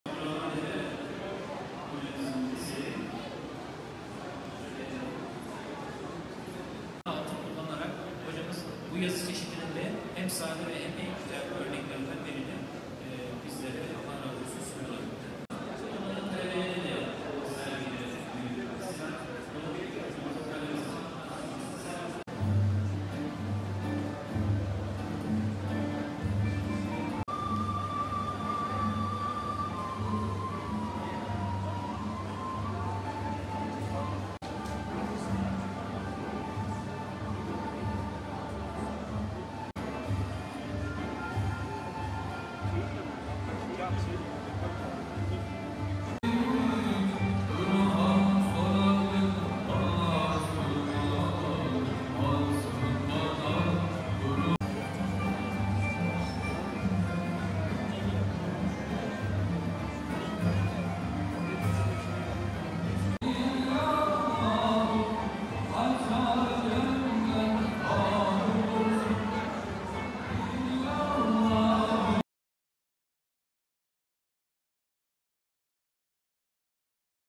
"Kelime-i Tevhid Hat Sergisi" sanatseverlerle buluştu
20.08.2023
Konya'da hat tablolarından hazırlanan "Kelime-i Tevhid Hat Sergisi" ziyarete açıldı.